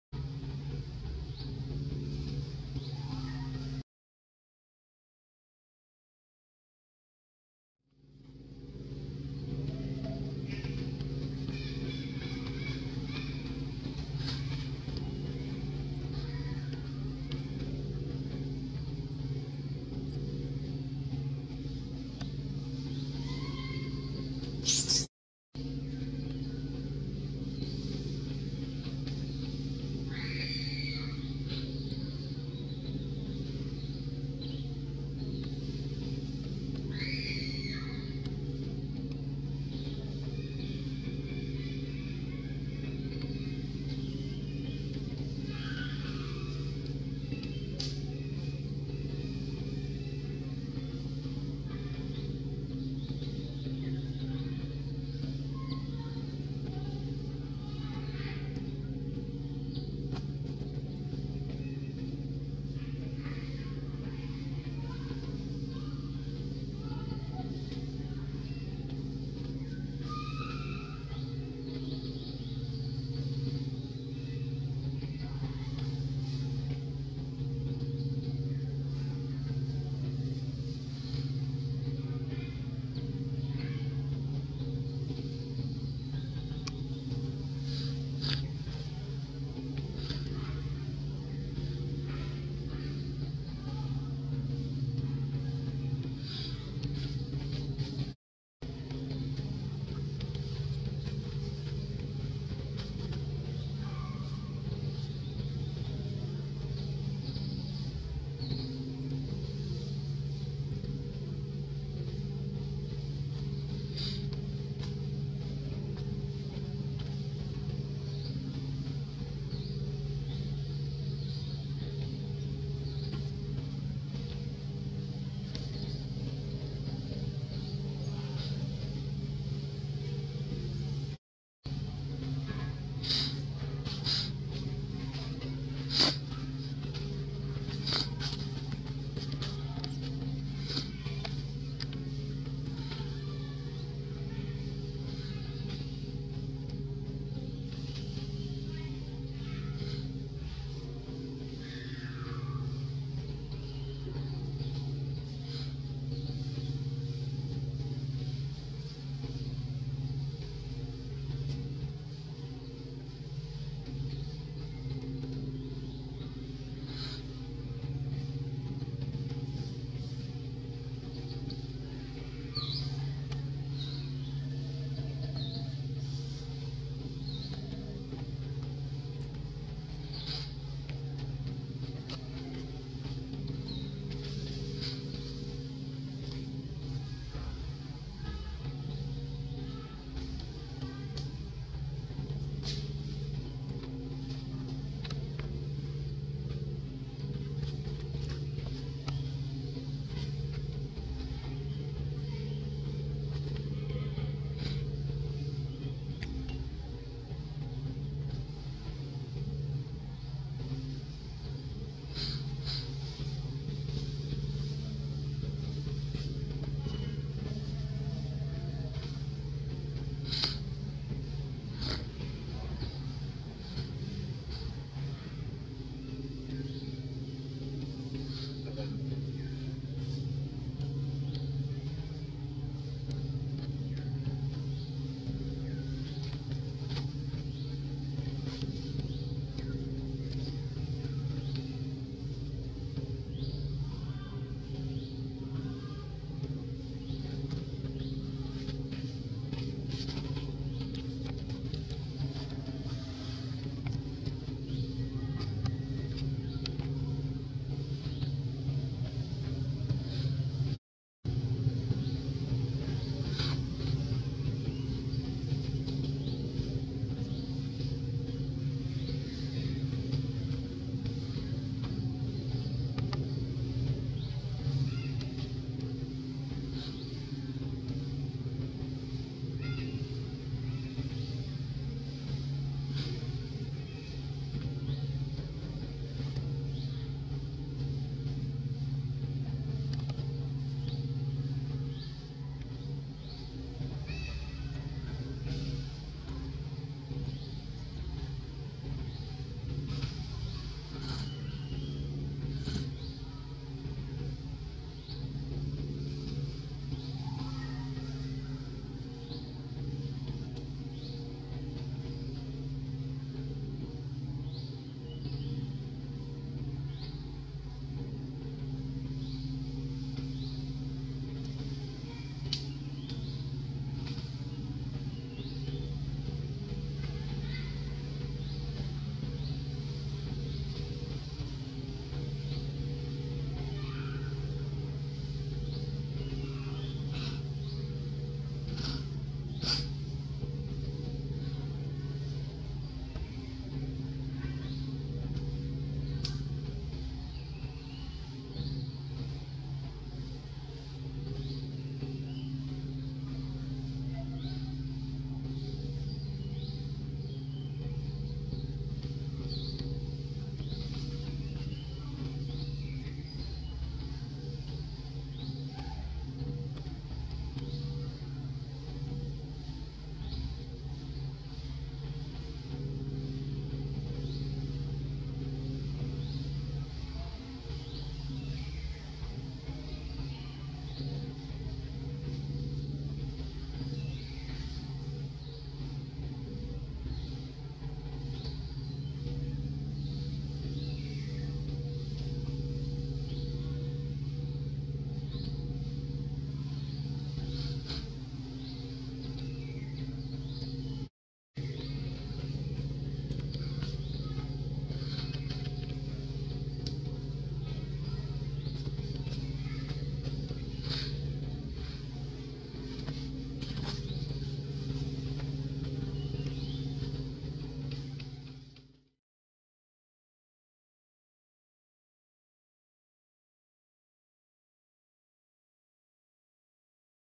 AMAZING !! POOR NEWBORN ANYA TRY HARD LEARNING WALK FOR MOM SEE, SEEM CONFIDENT NEWBORN MONKEY,
Dear all Value Visitor, This Videos We Want To Show You About ,AMAZING !! POOR NEWBORN ANYA TRY HARD LEARNING WALK FOR MOM SEE, SEEM CONFIDENT NEWBORN MONKEY,